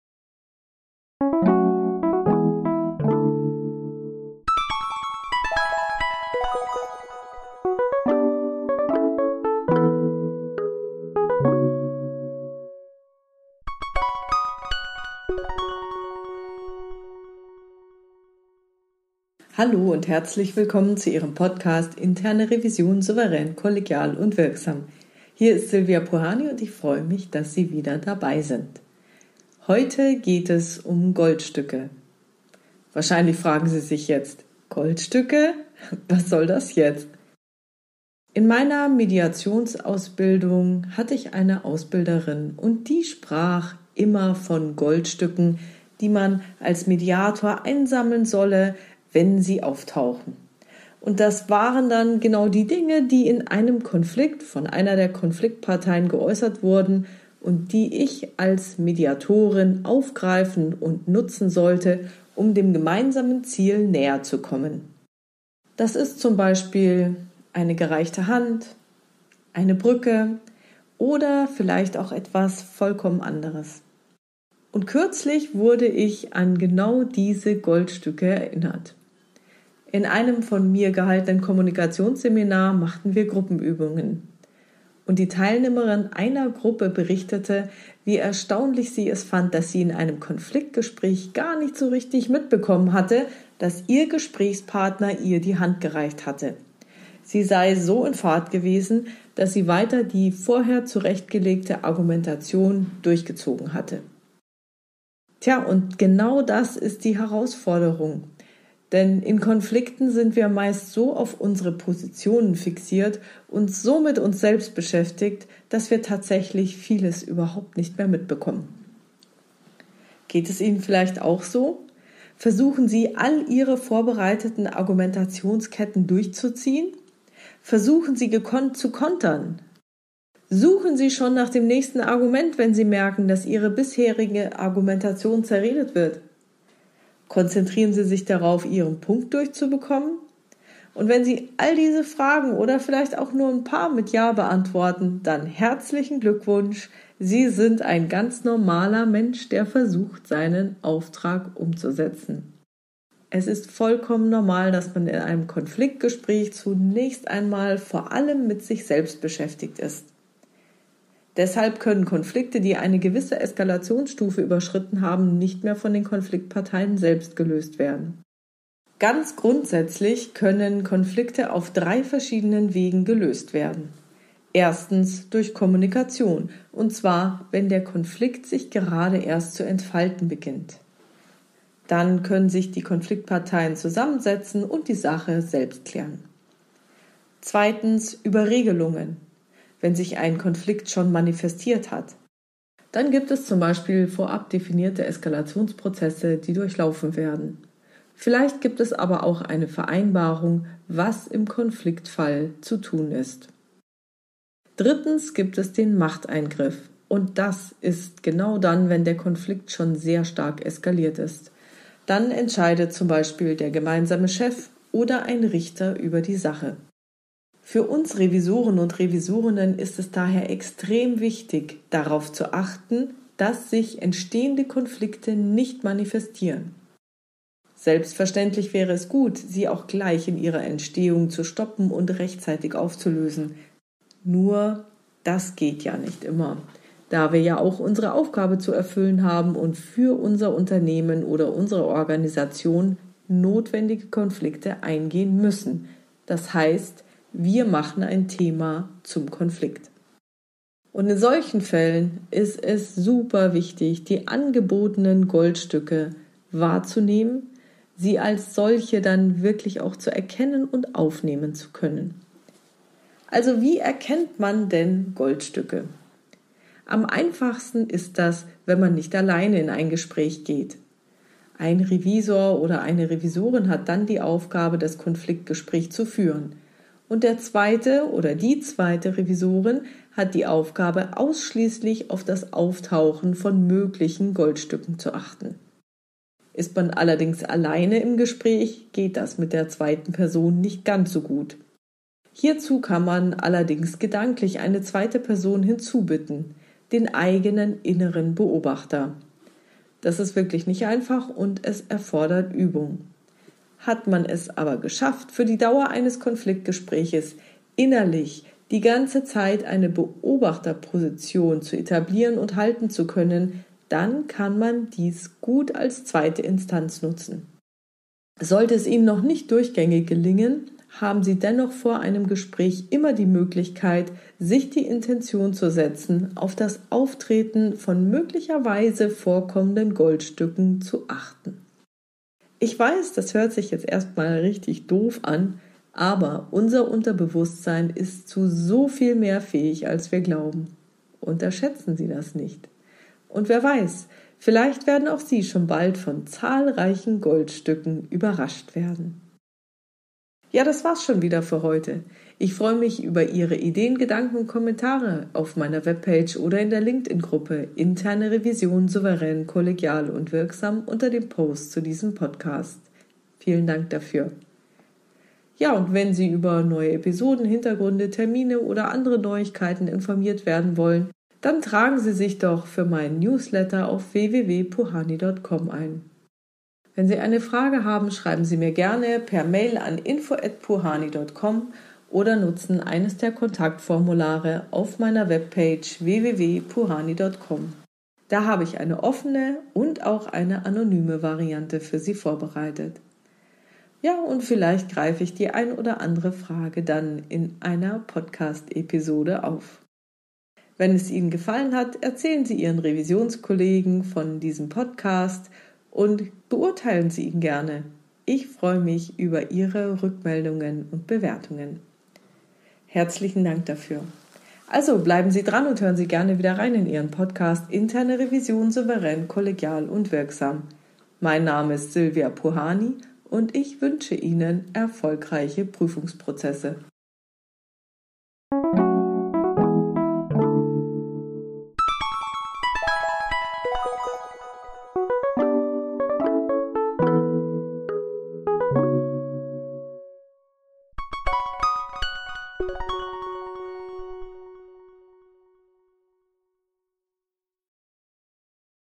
Folge 257: Wie Sie Goldstücke einsammeln
In einem von mir durchgeführten Kommunikationsseminar machten wir Gruppenübungen. Eine Teilnehmerin einer Gruppe berichtete in der Reflexion, wie erstaunlich sie es fand, dass sie in dem Konfliktgespräch gar nicht mitbekommen habe, dass ihr Gesprächspartner ihr die Hand gereicht habe. Sie sei so in Fahrt gewesen, dass sie weiter die vorher zurechtgelegte Argumentation durchgezogen habe.

In Konflikten sind wir meist so auf unsere Positionen fixiert und so mit uns selbst beschäftigt, dass wir tatsächlich Vieles nicht mehr wahrnehmen.

Geht es Ihnen auch so?
Versuchen Sie, Ihre vorbereiteten Argumentationsketten fortzuführen und Einverständnis zu Ihren Punkten zu erringen?
Suchen Sie schon nach dem nächsten Argument, sobald Sie merken, dass ihre bisherige Argumentation zerredet wird?

Wenn Sie eine oder auch alle diese Fragen mit „ja“ beantwortet haben, bestätigt dies, dass Sie ein ganz normaler Mensch sind, der versucht, seinen Auftrag umzusetzen.

Das ist die Herausforderung, um die es in diesem Podcast geht:
Wie realisiert man in Echtzeit, dass einem die Hand gereicht wird?

Ich wünsche Ihnen viel Spaß beim Zuhören und erfolgreiche Prüfungsprozesse!